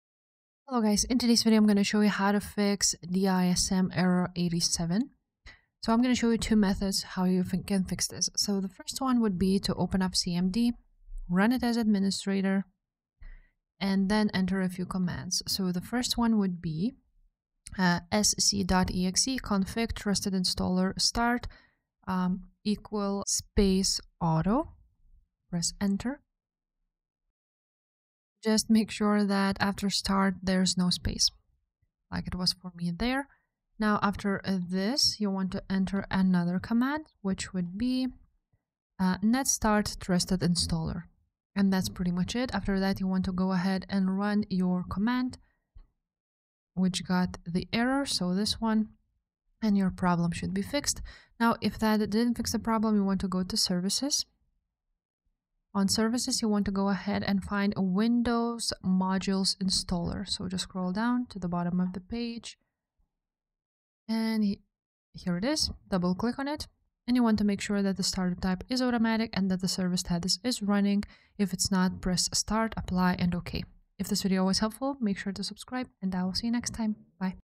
How To Fix DISM Error 87 The Easy Way
In this video I show you How To Fix DISM Error 87 The Cleanup-Image Option is Unknown!